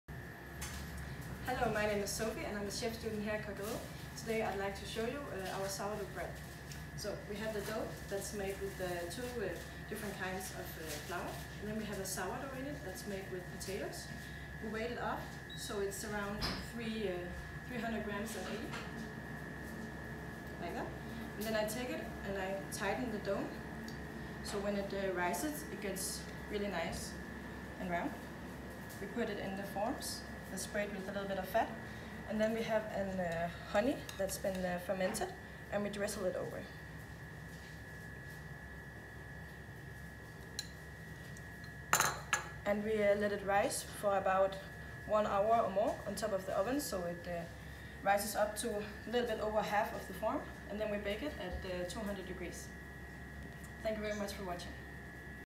See how we get our sourdough bread ready
Here is our student chef Sofie telling you a bit about our sourdough bread